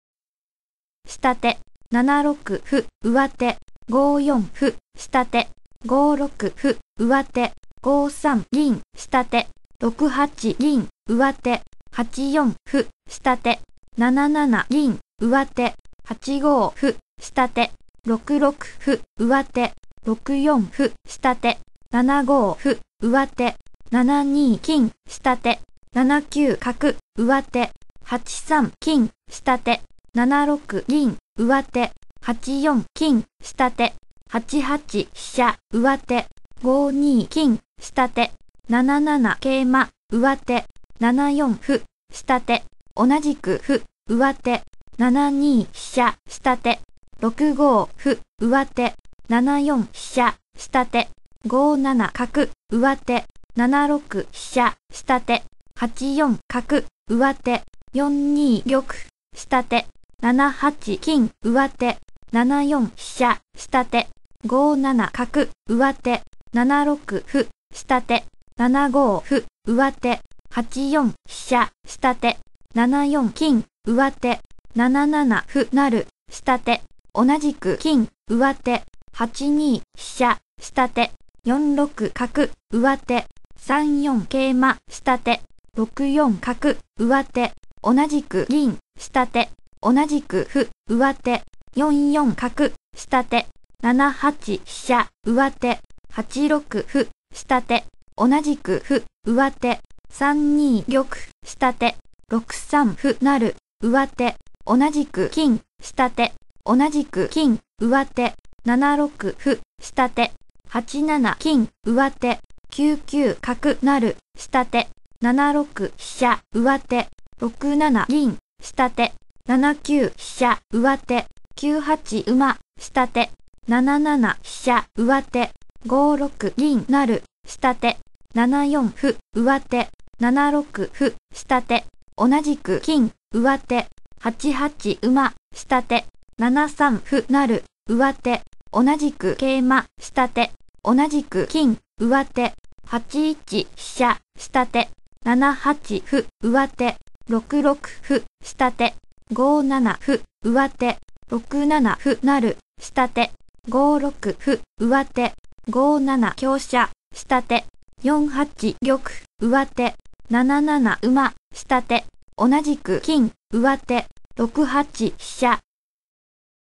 将棋 大橋宗古1635 二十二 ▲左海久円VS▽ 勝 大橋宗古
寛永十二年
角行落
まで七十九手で宗古の勝ち
あっけない幕切れ
出典　日本将棋大系1　筑摩書房